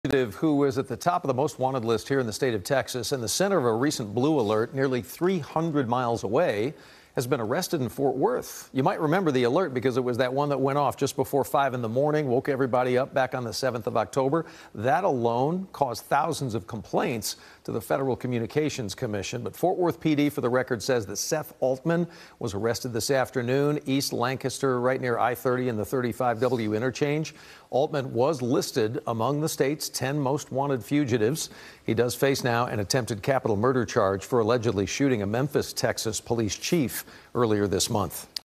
Top 10 Most Wanted fugitive arrested in Fort Worth for alleged police shooting
Seth Altman, a fugitive on Texas’ 10 Most Wanted list, was arrested in Fort Worth for allegedly shooting a police chief in Memphis, Texas. A Blue Alert issued on Oct. 7 was related to this case.